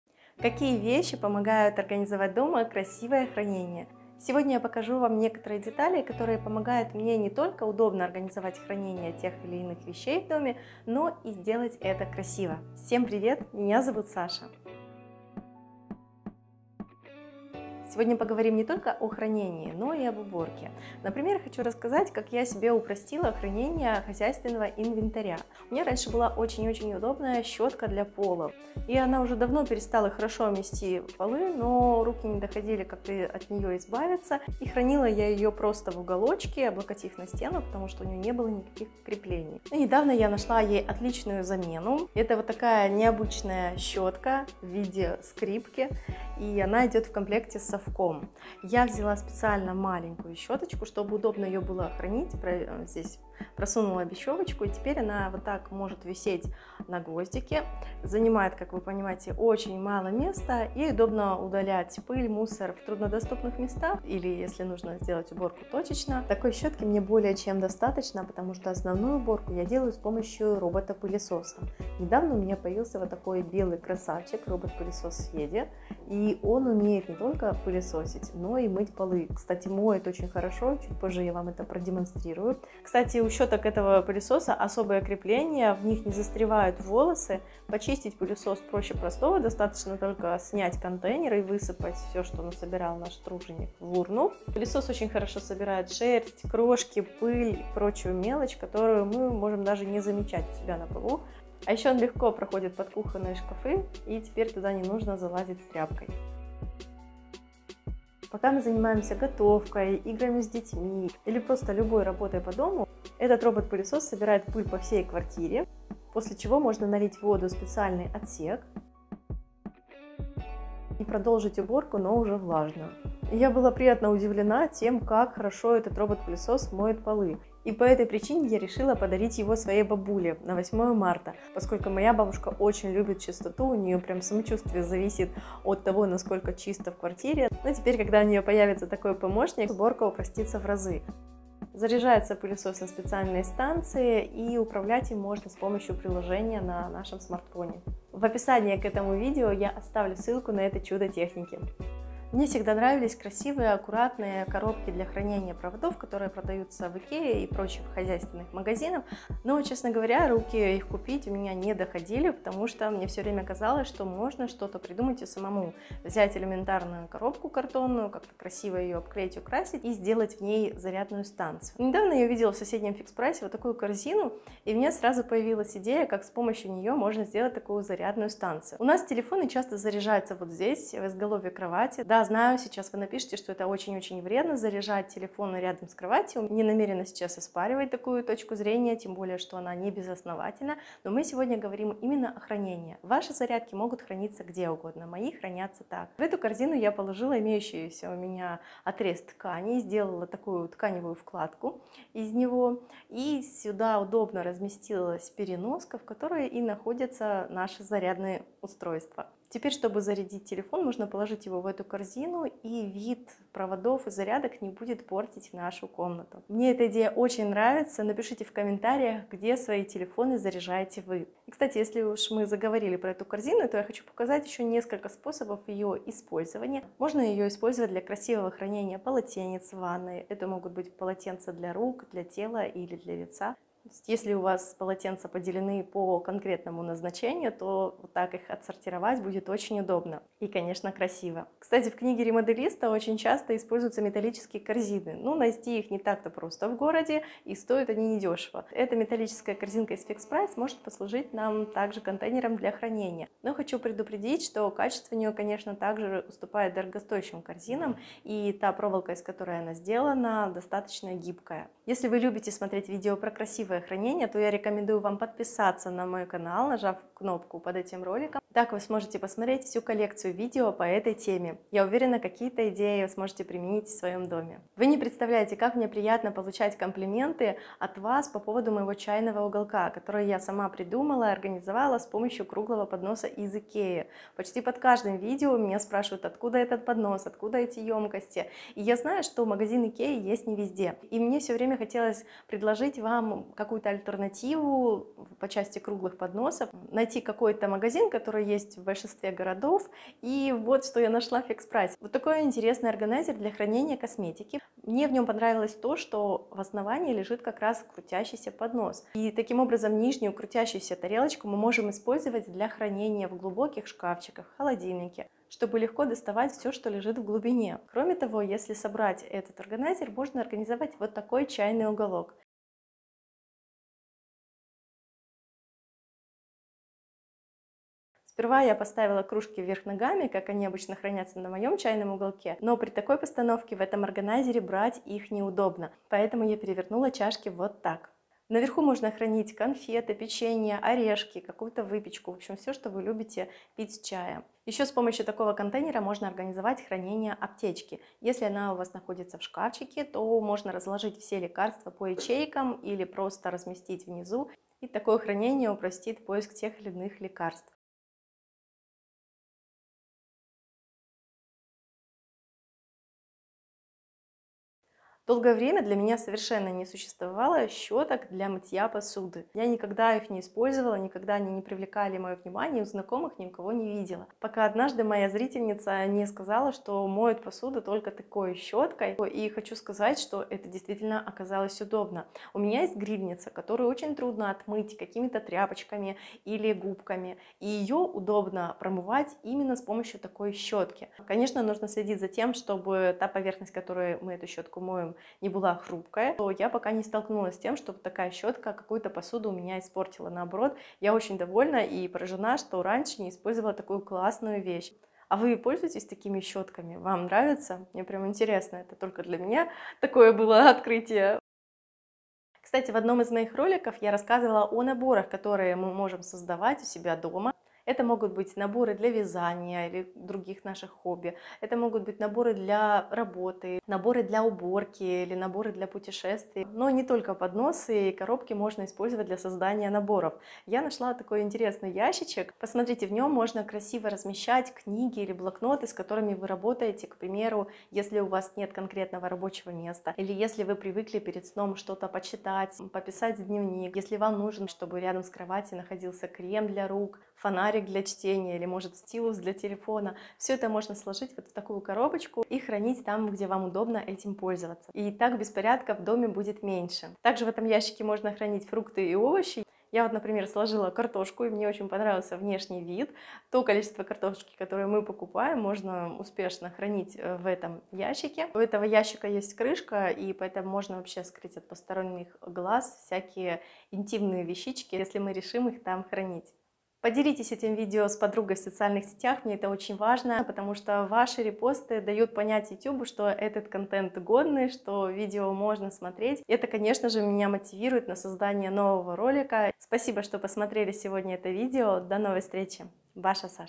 Органайзеры из Фикспрайс, Озон и Алиэкспресс для хранения и уборки
✅ Показываю идеи для хранения вещей и упрощения уборки. Для этого на помощь придут органайзеры из Фикспрайс, Озон, WILDBERRIES и Алиэкспресс. Давайте займемся организацией пространства дома и будем хранить красиво наши вещи, а заодно посмотрим покупки из фикспрайс для хранения.

Робот-пылесос Yeedi k650

YEEDI650M5 $10 OFF